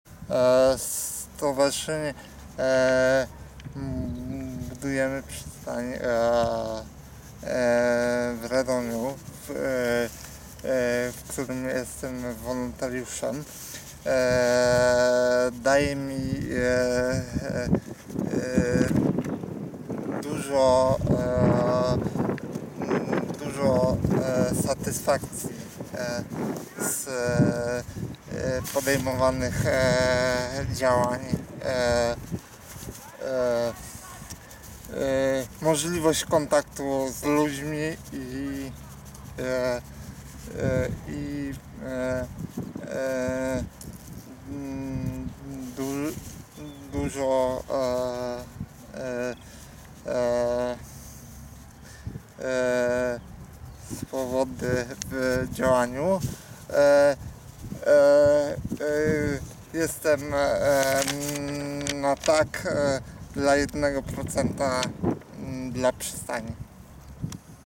Przyjaciele o Przystani - Wojciech Stanios
"Stowarzyszenie Budujemy Przystań daje mi dużo satysfakcji z podejmowanych działań, możliwość kontaktu z ludźmi i dużo swobody w działaniu. Jestem na TAK dla 1% dla Przystani!", mówi nasz wolontariusz Wojciech Stanios. Bez Wojtka wiele naszych działań byłoby bardzo trudnych do zrealizowania. Dziękujemy za Twoje zaangażowanie i dobre rady! :)